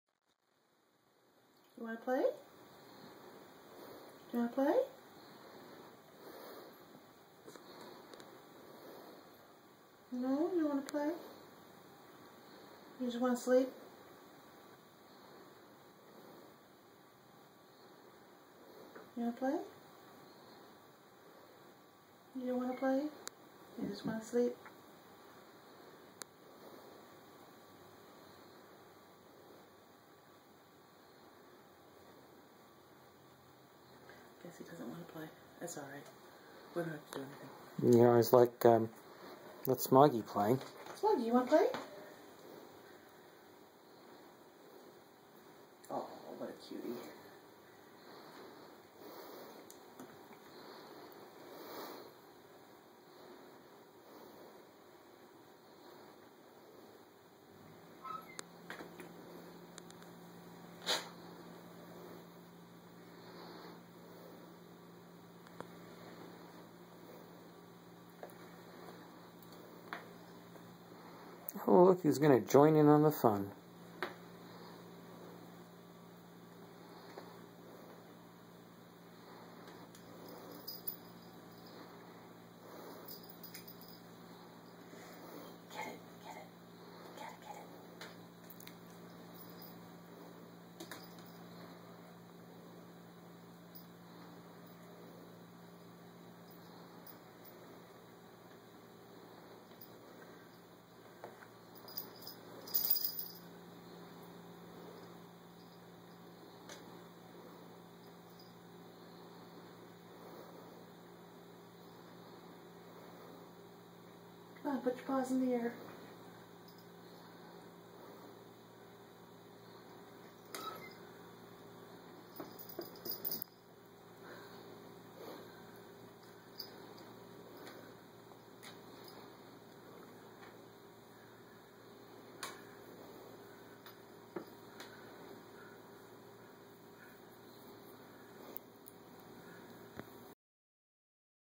Willowby, Smaug and Sally
Just some of our cats playing.